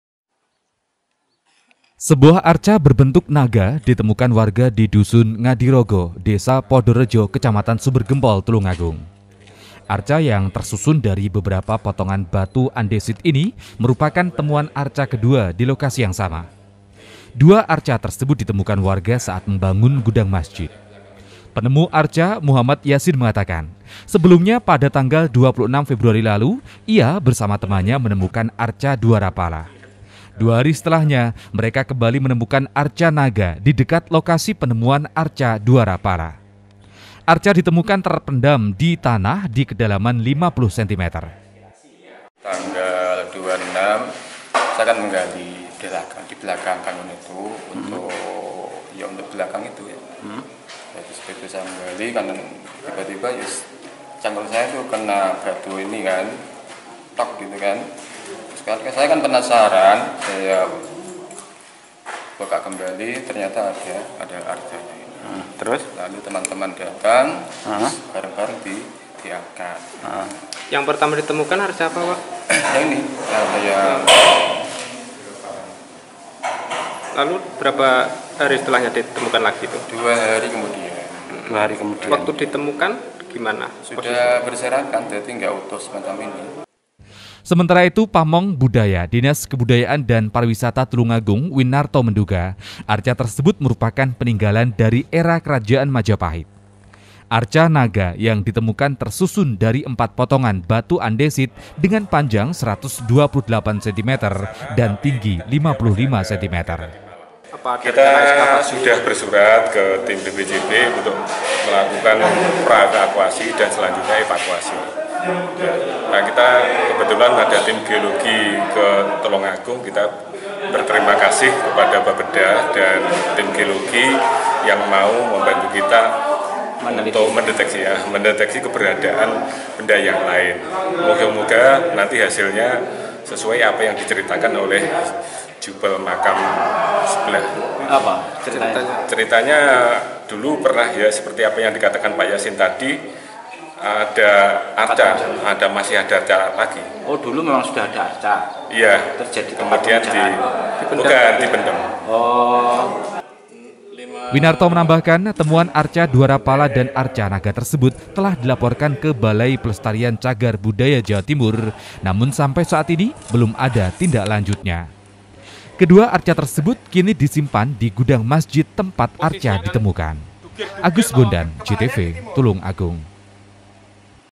TULUNGAGUNG - Warga Kembali Temukan Arca Naga Di Dekat Lokasi Penemuan Arca Dwarapala
Setelah penemuan arca dwarapala di tulungagung beberapa waktu lalu  warga kembali menemukan arca berbentuk naga di lokasi yang sama. Penemuan benda diduga cagar budaya tersebut telah dilaporkan ke balai pelestarian cagar budaya jawa timur namun belum ada tindak lanjut. Kedua arca tersebut kini disimpan di gudang masjid .